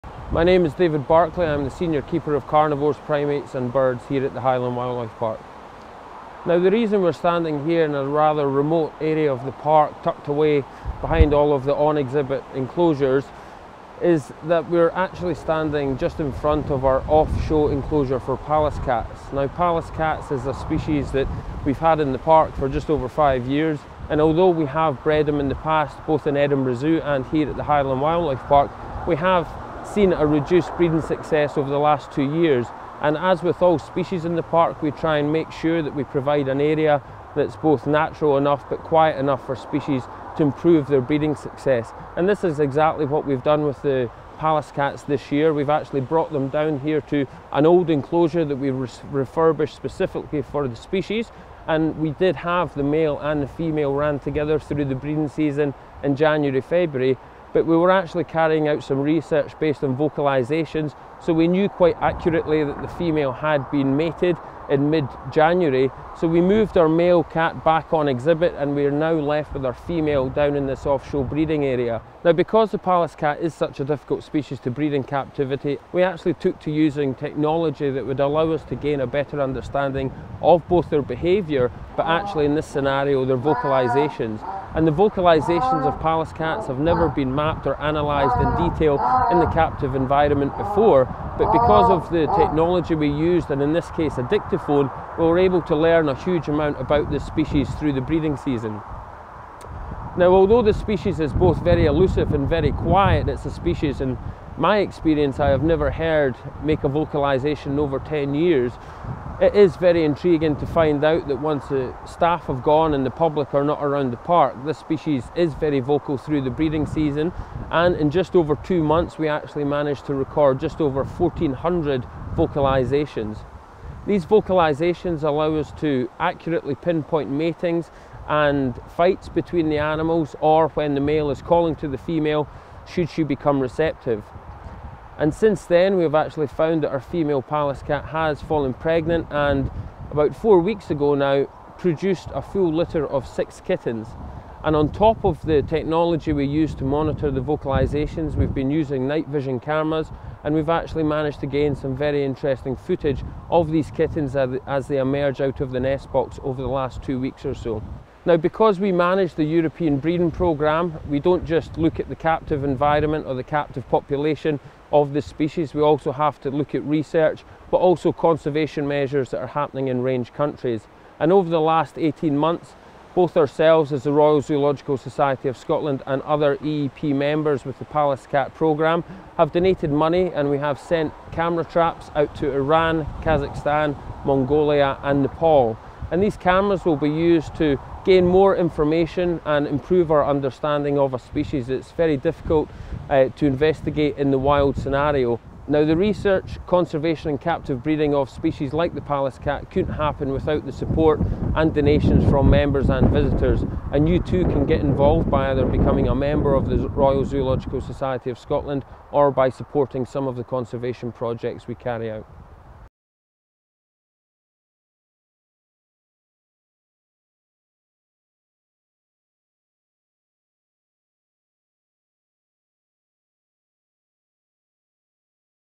Pallas Cats - RZSS Conservation Projects - Great days out for a good cause
Although they are a fairly common species in zoos, very little is known about the Pallas's cat (Otocolobus manul) in the wild.
David Barclay, one of the senior keepers at the Highland Wildlife Park, manages the European breeding programme for Pallas's cat and maintains the International Studbook which records all captive Pallas's cats anywhere in the world. Although only classed as Near Threatened, one of the low risk threat categories, we have seen how the status of a species that was long considered common can become highly threatened in a very short period of time.

Through his contacts within the cat conservation community, especially the Pallas's Cat Working Group, he became aware of field researchers in Iran and Nepal that were studying Pallas's cats but had little equipment. He then contacted all the participants of the breeding programme asking for donations to buy and supply video-trap cameras to the Iranian and Nepalese researchers. Many of the various zoos, along with us, helped out and the equipment has now been dispatched to the relevant researchers for placement in the field.

This work will not only increase our understanding of the species globally but it will highlight key areas that will allow for better targeting of conservation efforts in range countries and at the same time aid our efforts with the captive breeding programmes.